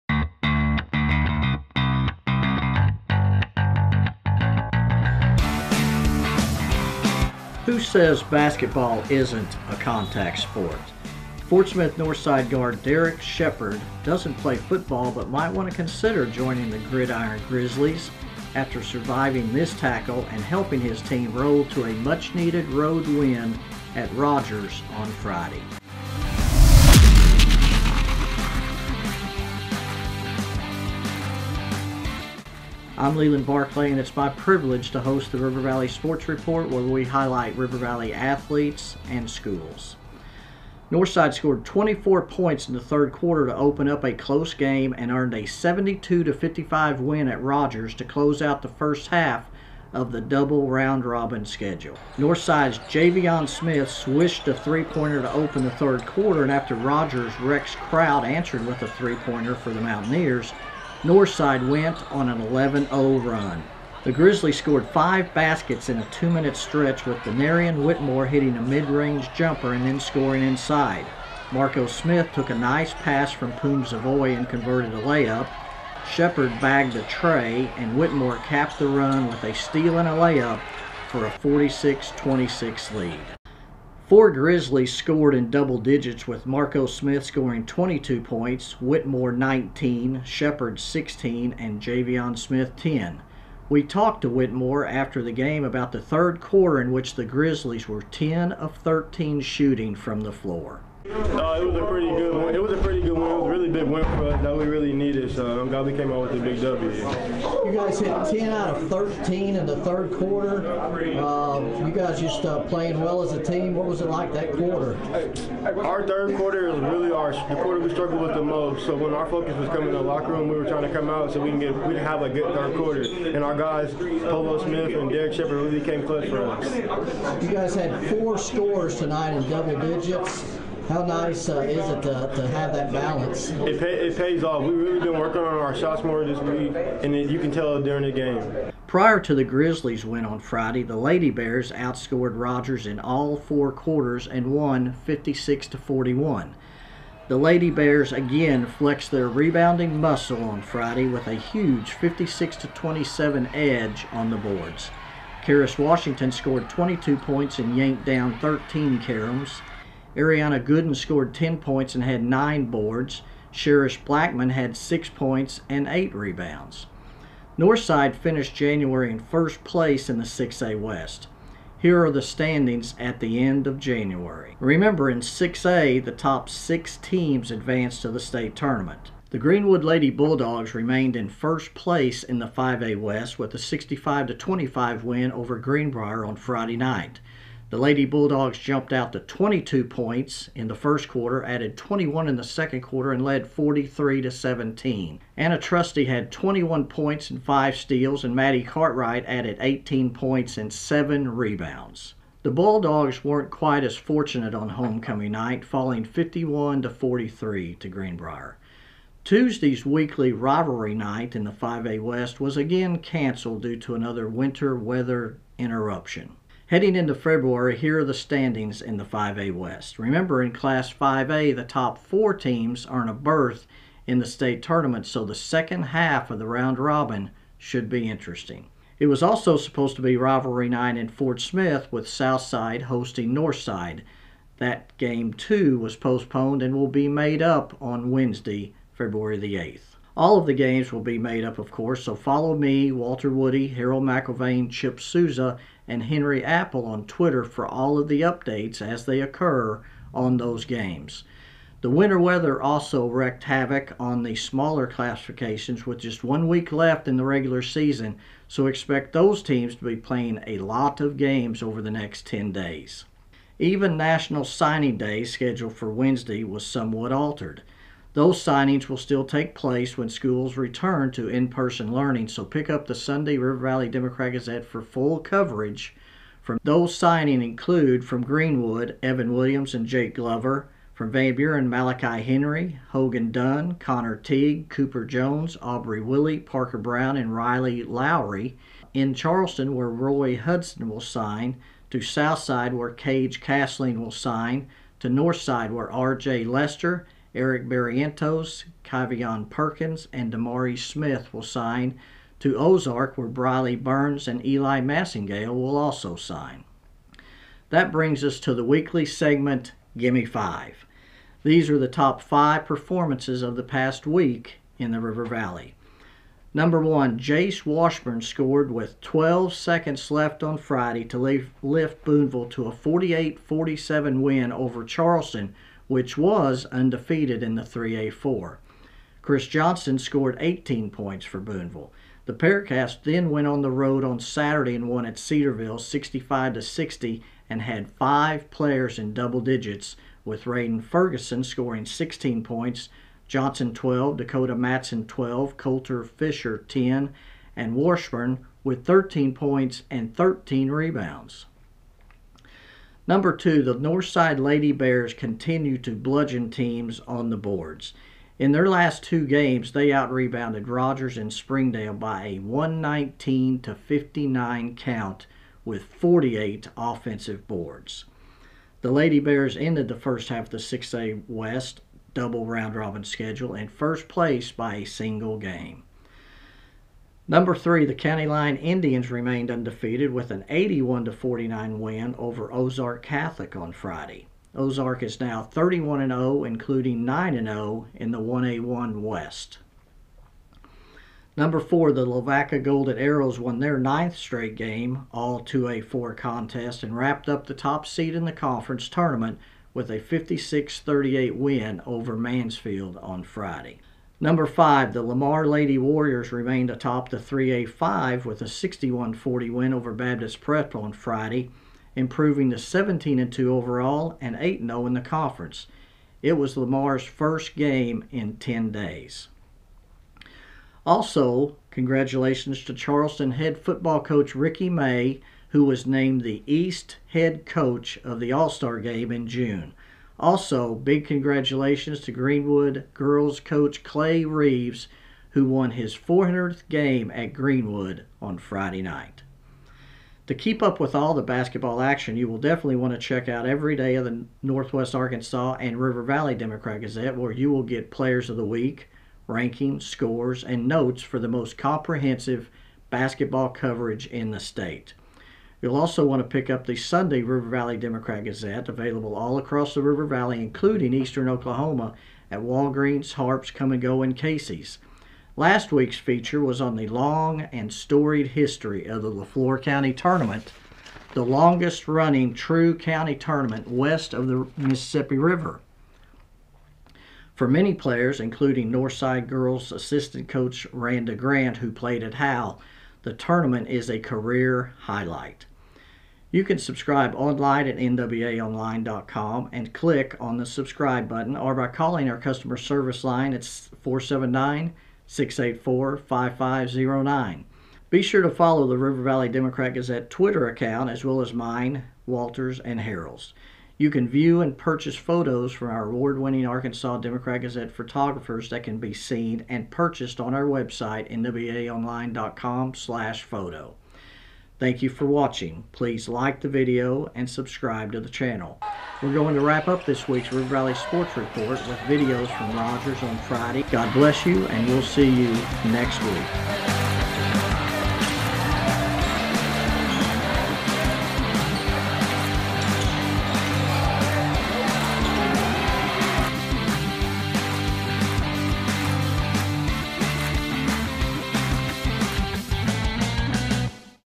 River Valley Report - February 1, 2023
With all the area's basketball games postponed this week due to winter weather, Leland Barclay makes up for it by recapping this past Friday's action — including Fort Smith Northside's sweep at Rogers and the Greenwood Lady Bulldogs' homecoming thrashing of Greenbrier. That's followed by a quick update of the National Signing Day's postponed events and the latest installment of GIMME 5.
(Video produced by Hank Layton / River Valley Democrat-Gazette)